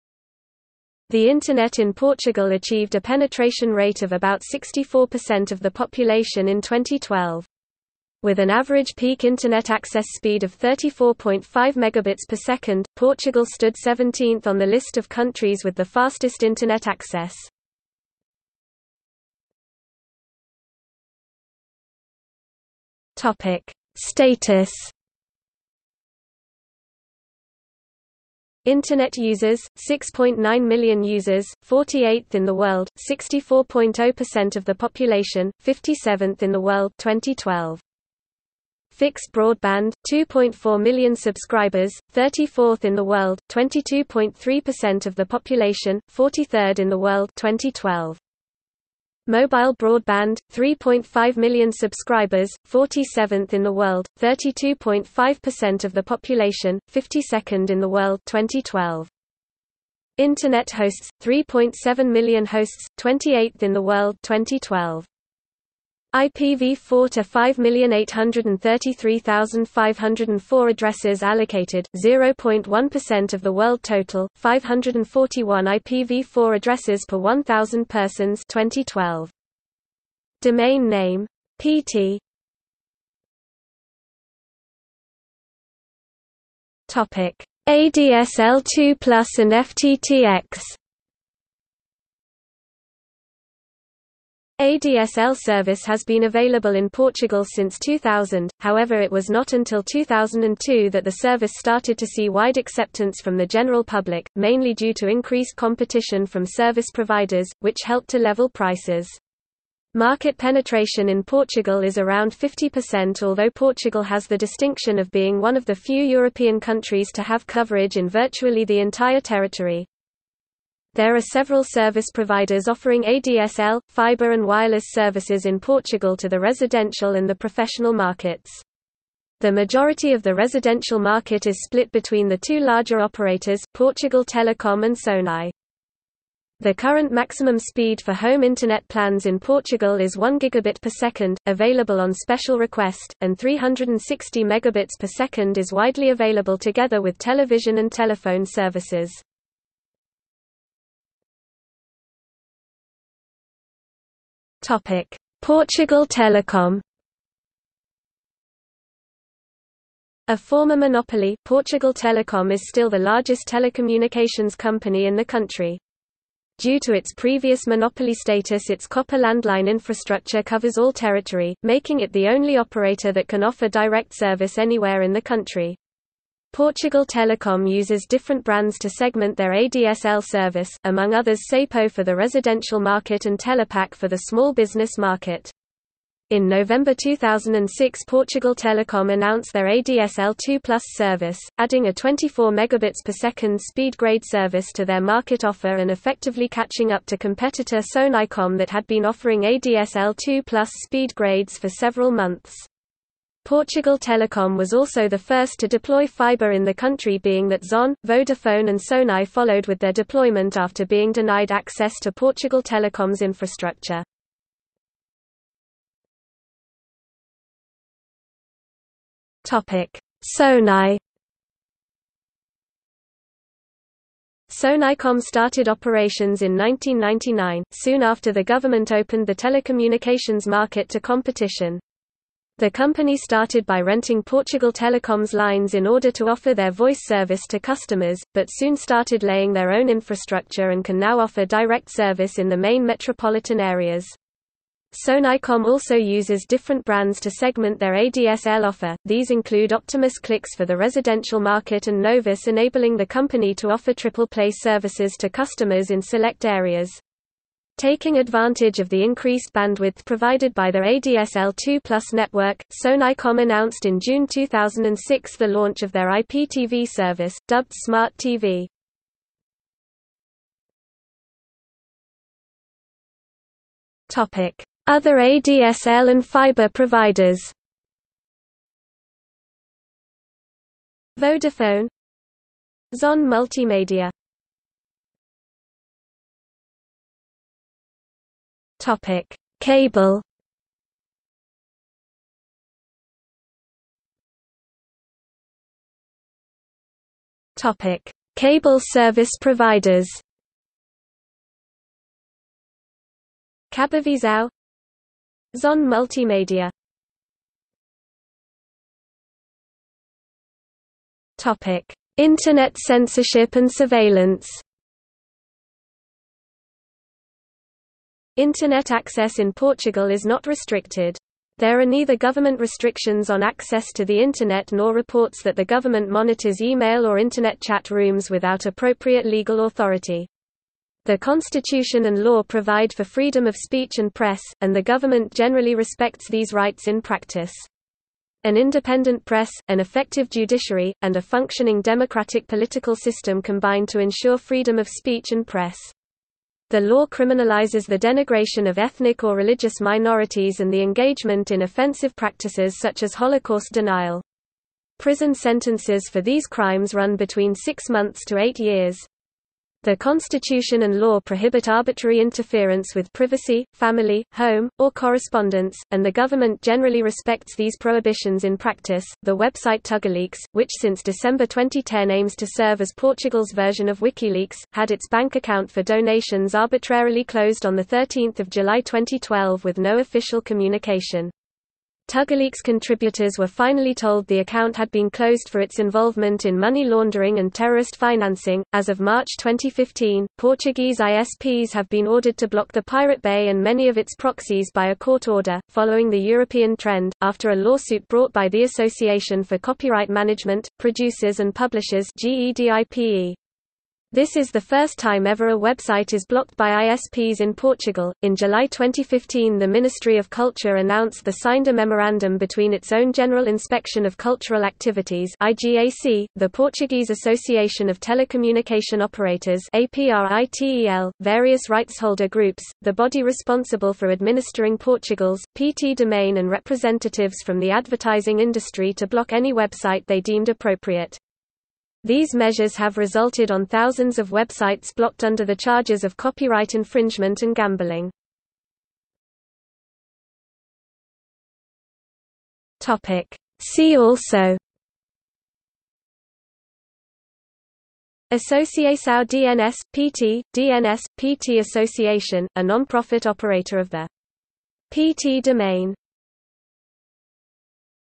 Internet in Portugal | Wikipedia audio article
00:00:19 1 Status
00:01:23 2 ADSL2+ and FTTx
00:02:31 2.1 Portugal Telecom
00:03:36 2.2 Sonae
00:04:30 2.3 Other ADSL and fiber providers
00:04:45 3 Cable
00:04:54 3.1 Cable service providers
00:05:07 4 Internet censorship and surveillance
00:07:19 5 See also

- Socrates

SUMMARY
The Internet in Portugal achieved a penetration rate of about 64% of the population in 2012. With an average peak Internet access speed of 34.5 megabits per second, Portugal stood seventeenth on the list of countries with the fastest Internet access.